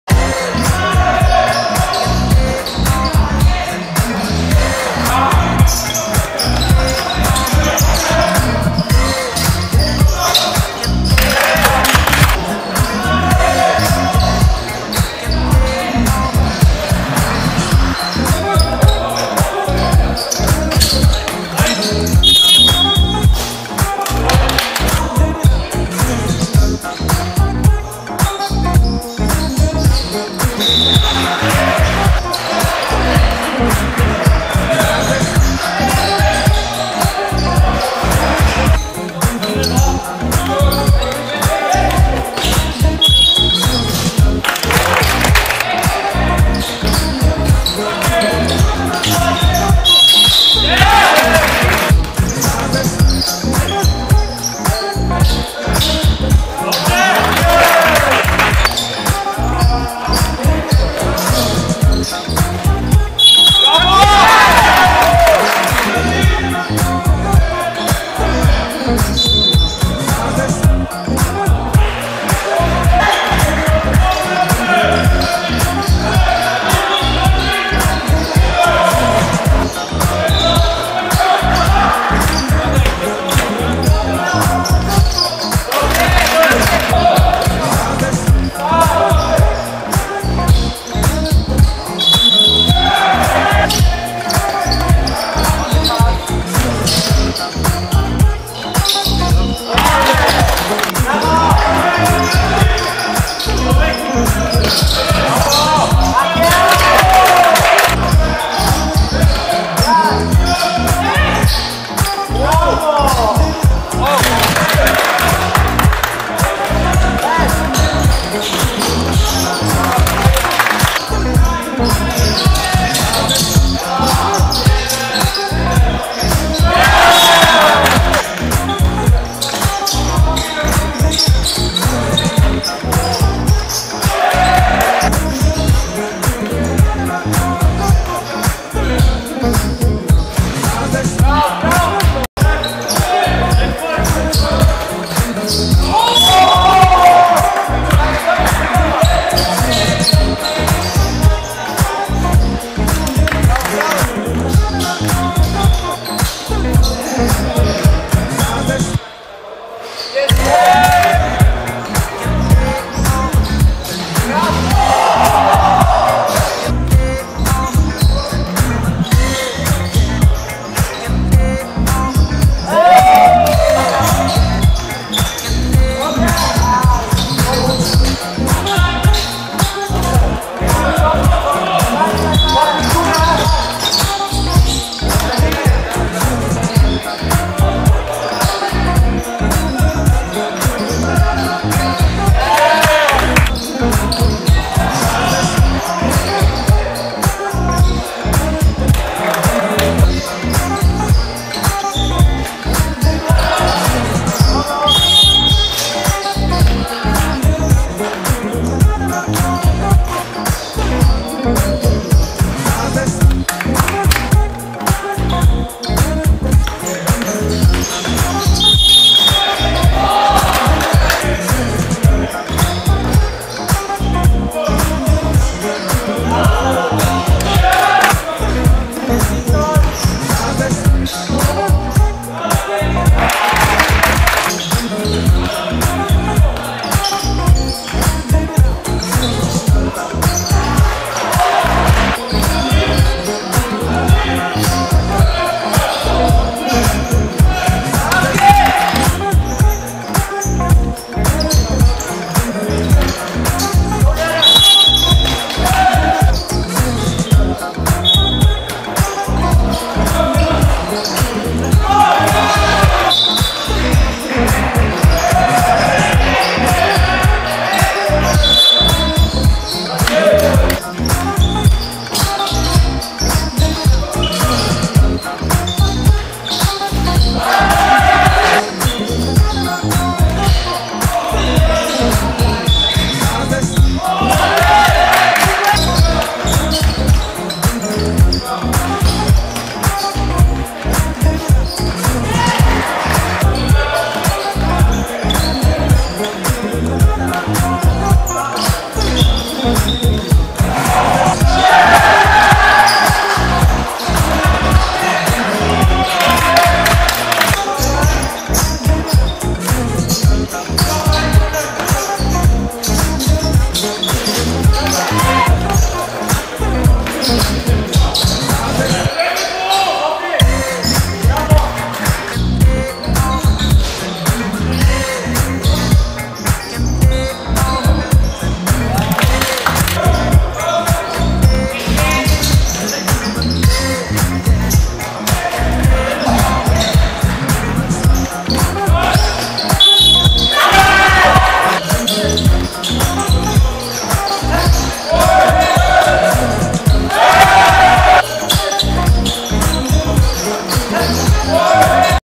13 02 Zefir vs Tajfun – Liga Wiatrów 25
ZEFIR 75:67 TAJFUN
M.Witkowski 30pkt 7-8ft 11zb 6ast 3stl
J.Jaworski 18pkt 14zb